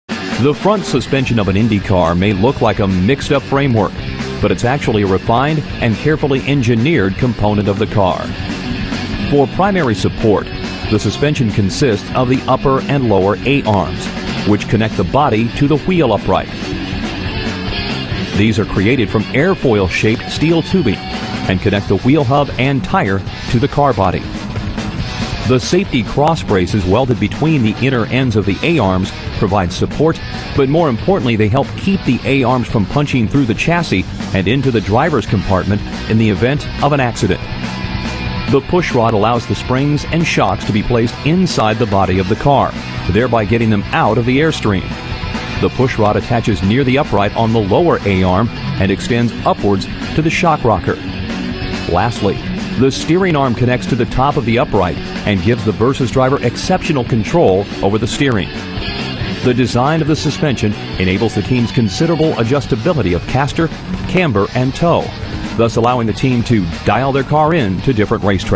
La sospensione anteriore di un autoveicolo
Video per la lezione ' SETUP autoveicolo '.
Ing. Mattia Strangi Facoltà di ingegneria meccanica di Bologna.